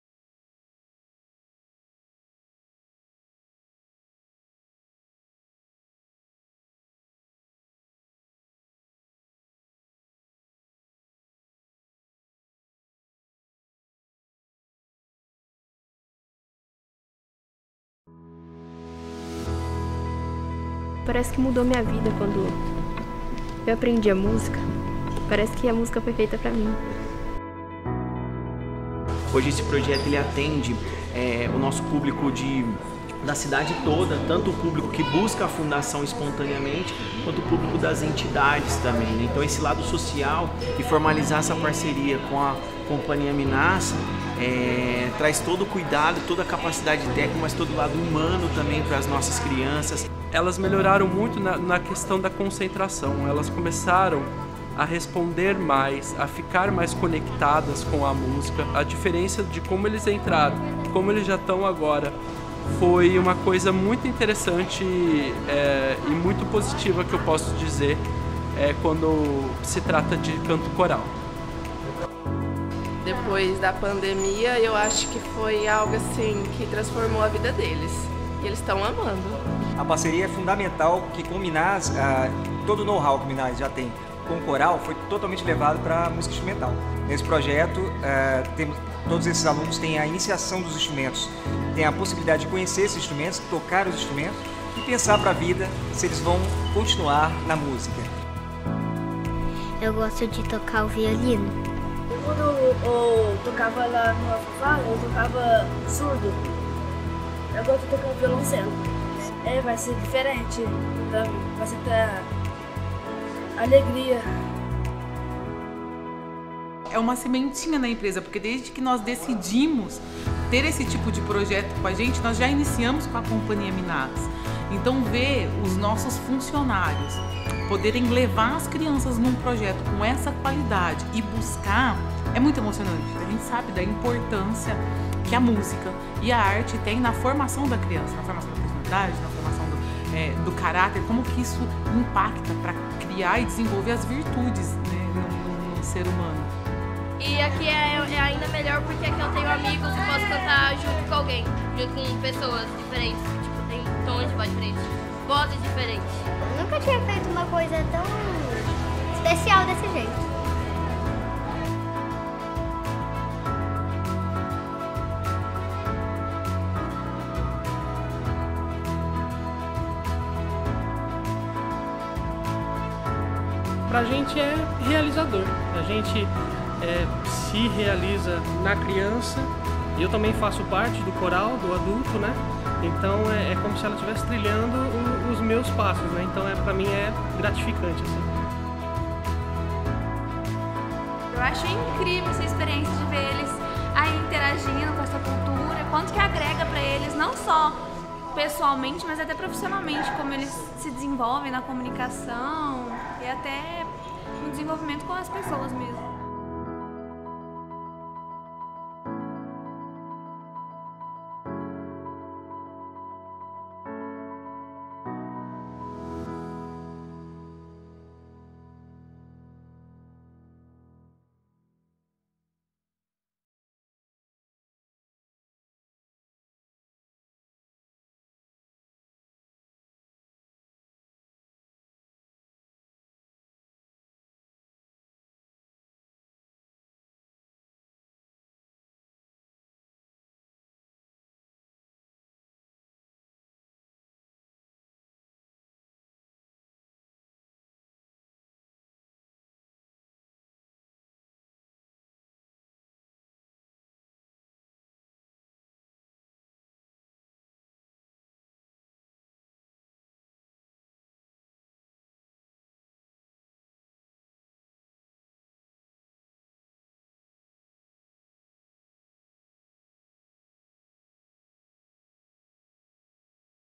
Projeto Cantar e Tocar - Semana Minaz de Corais 2022
Mais de 500 coralistas se encontraram na Semana Minaz de Corais, no Teatro Minaz.
A Semana Minaz de Corais divulga a música coral, através de apresentações de corais infantis, juvenis e adultos da Cia. Minaz e corais convidados.

Desde 2012 já foram apresentados inúmeros corais e realizadas oficinas e palestras com o intuito de intercambiar técnicas e repertórios e aprimorar os conhecimentos dos profissionais do canto coral e coralistas amadores.

Edição e Captação - Alexandre Galante Ocdy